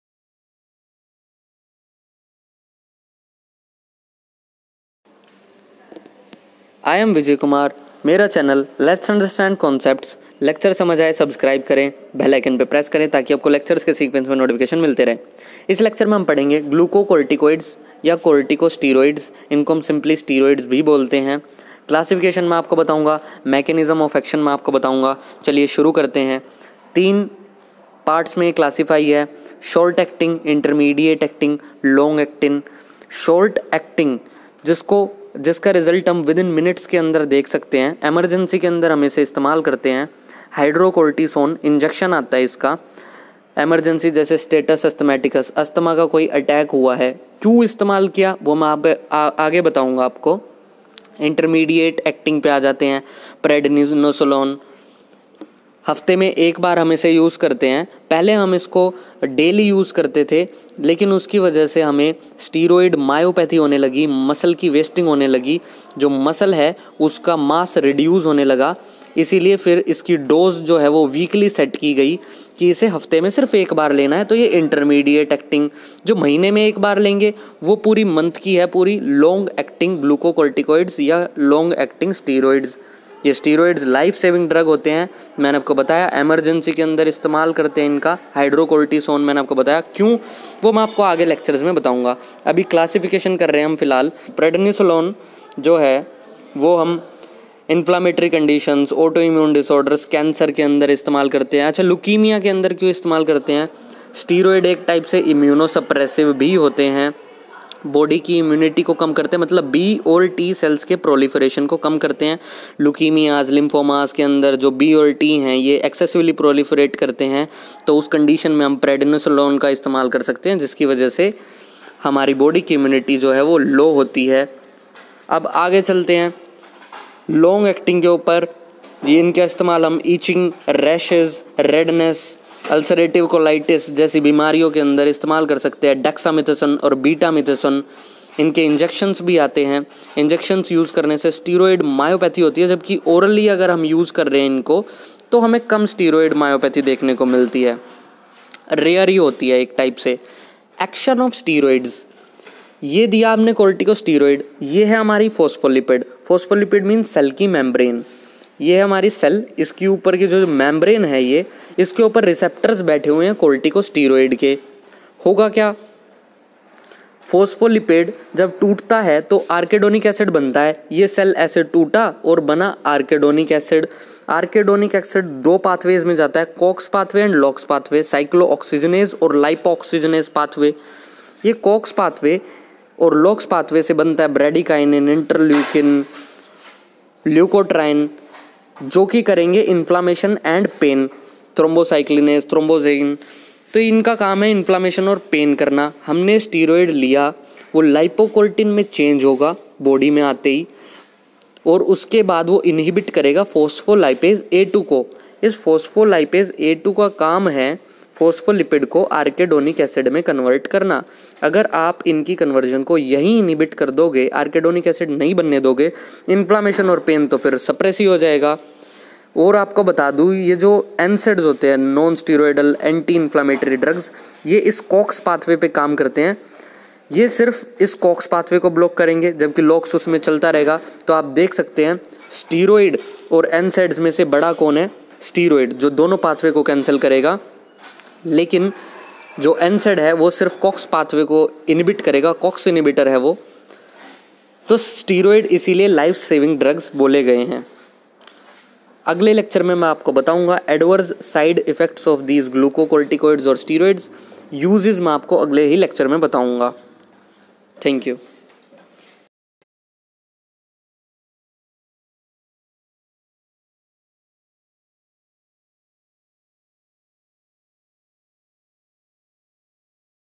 Glucocorticoids Classification | Glucocorticoids Mechanism Of Action | Corticosteroids Pharmacology
"Adverse Effects of Steroids " in next lecture...

"any query in this lecture comment it below"